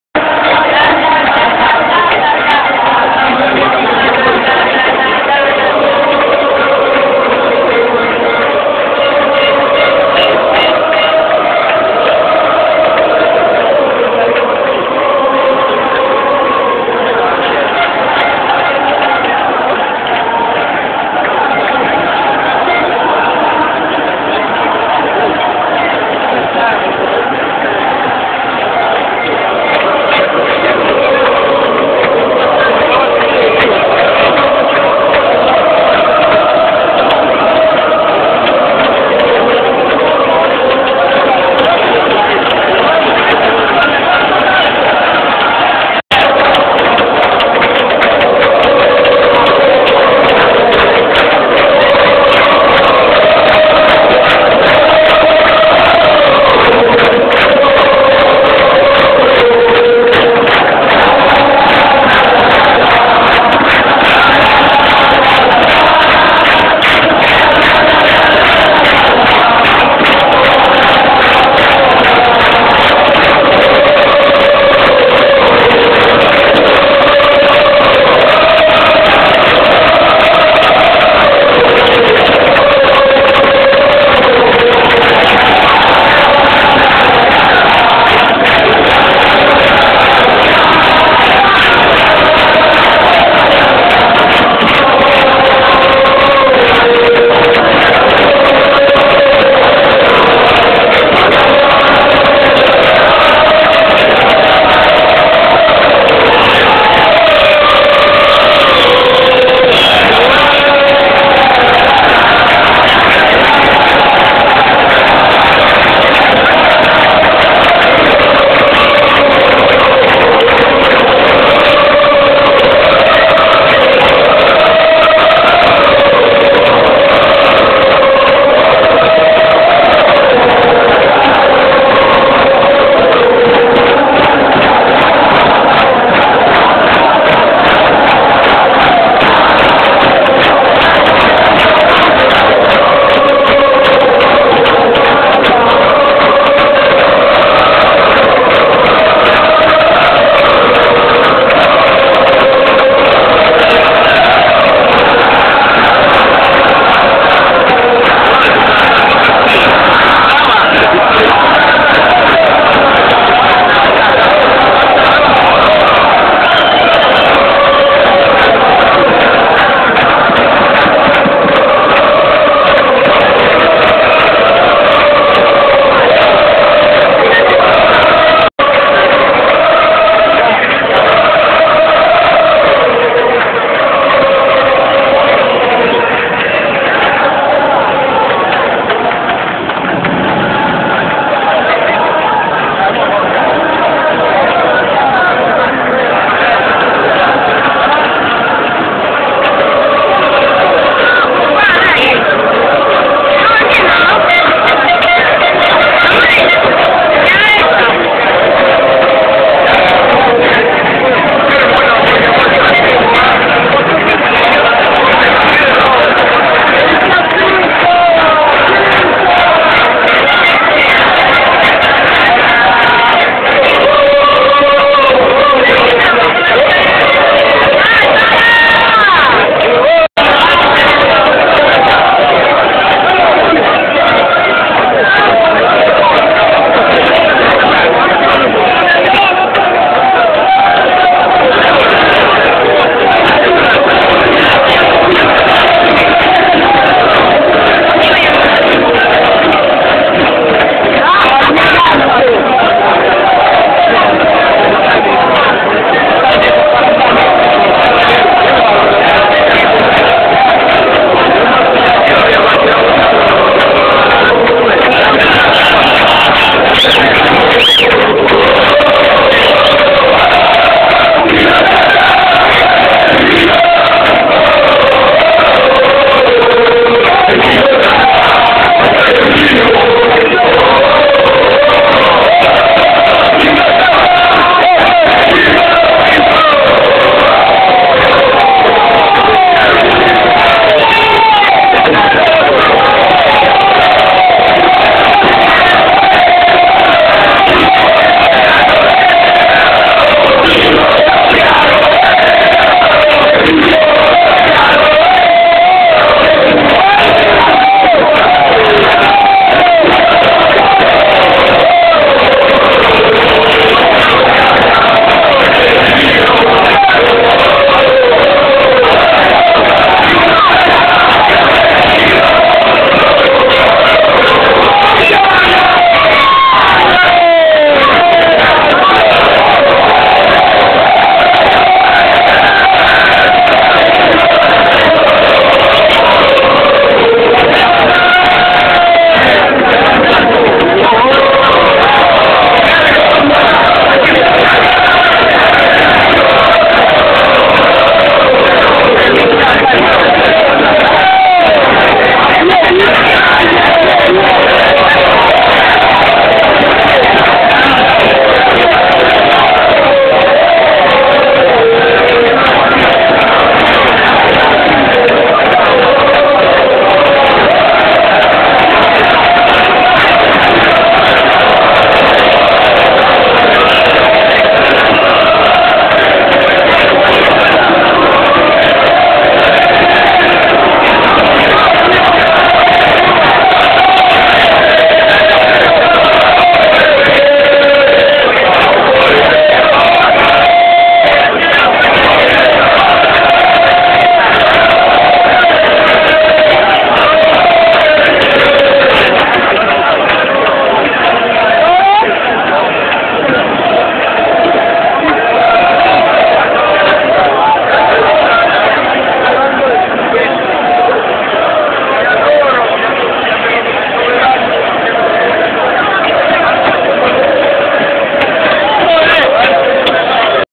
Ternana promossa in B: l'uscita del pulman delle fere!!!
Ternana promossa in serie B: ecco l'uscita delle FERE dallo stadio!